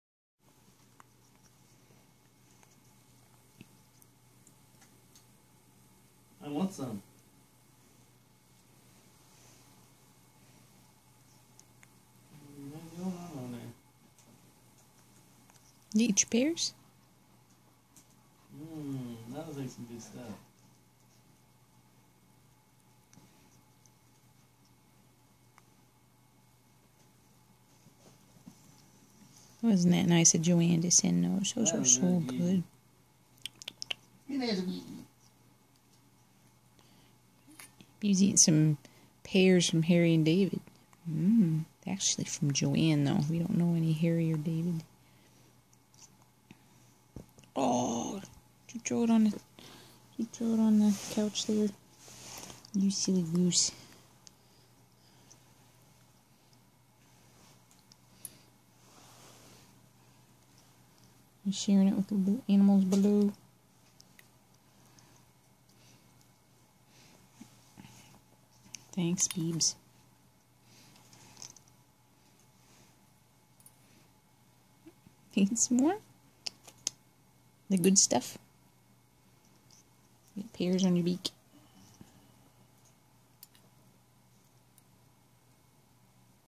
African Grey Parrot Bibi - Pears!
Mmmm! Some fresh Harry & David pears. Good stuff!